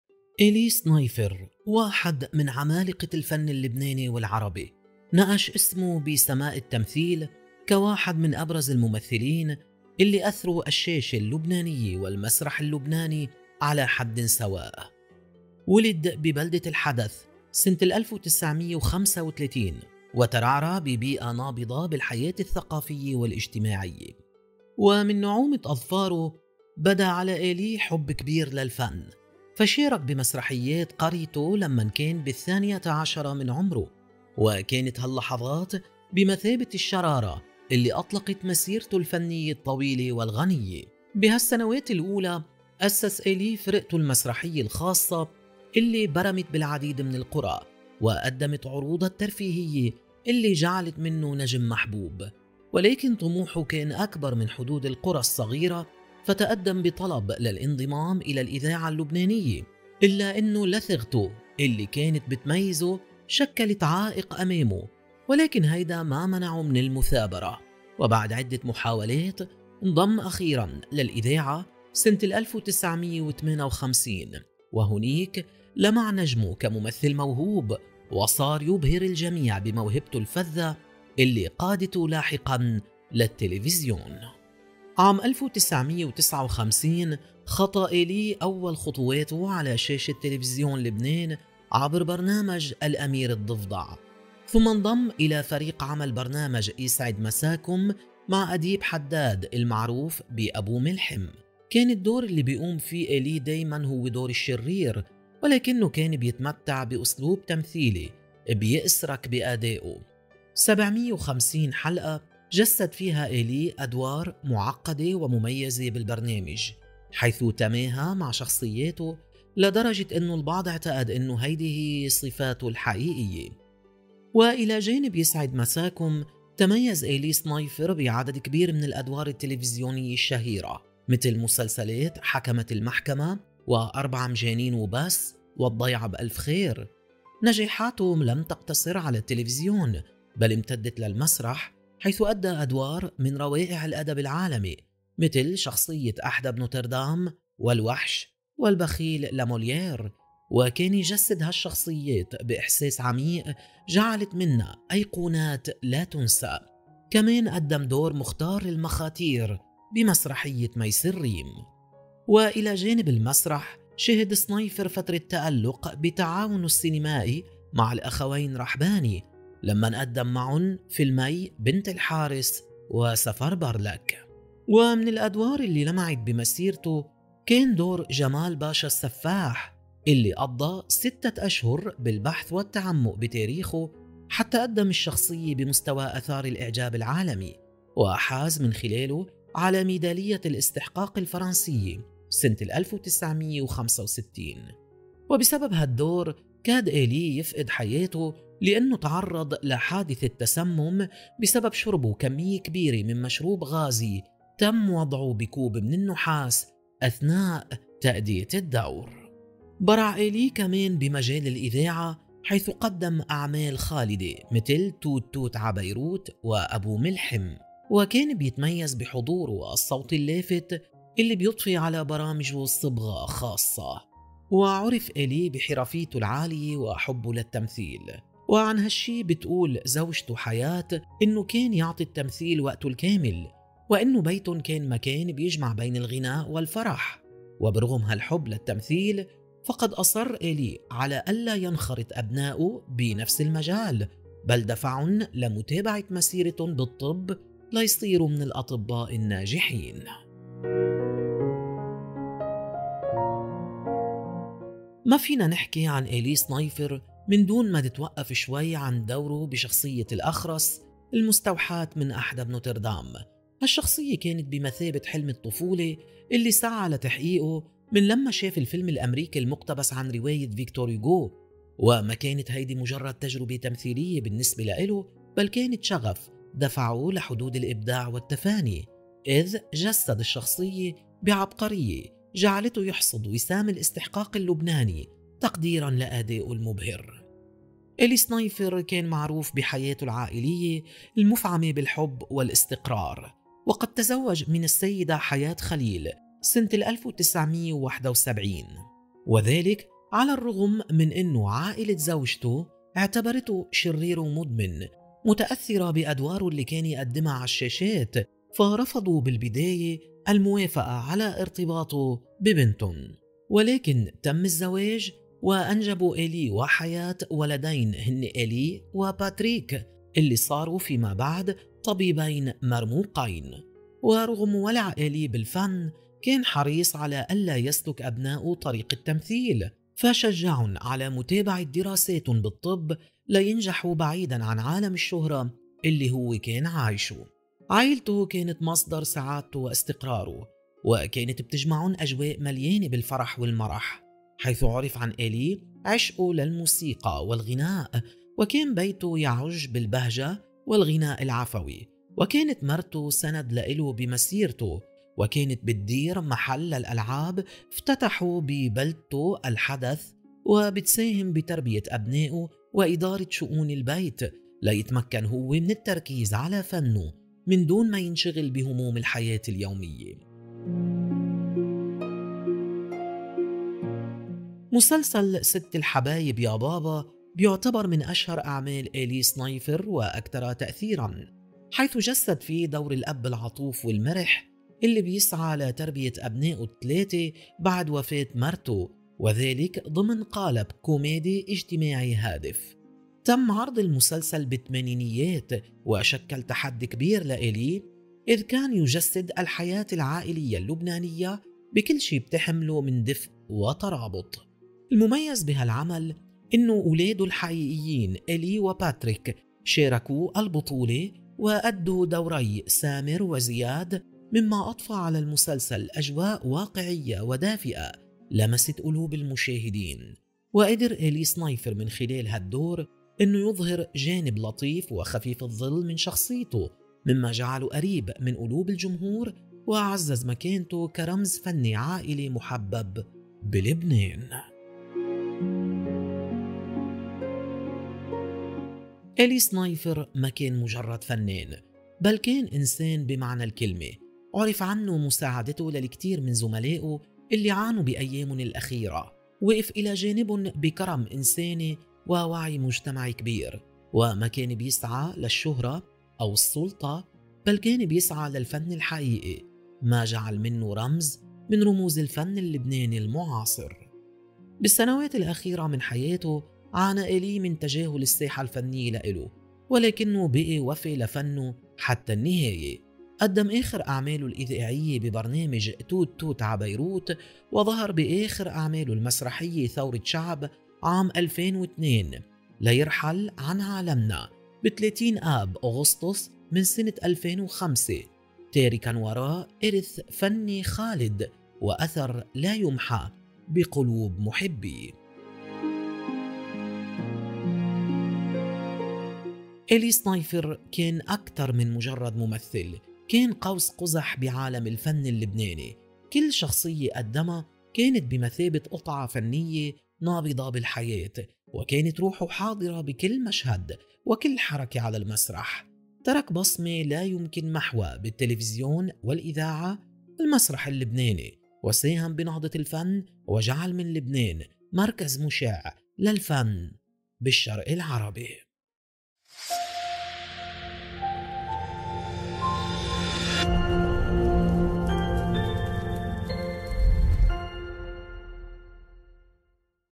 قصة حياة ايلي صنيفر لماذا رفضه أهل زوجته؟ وكيف اصبح شكل اولاده بعد 35 سنة على ظهورهم في مسلسل معه؟
إشترك في قناتنا على تلغرام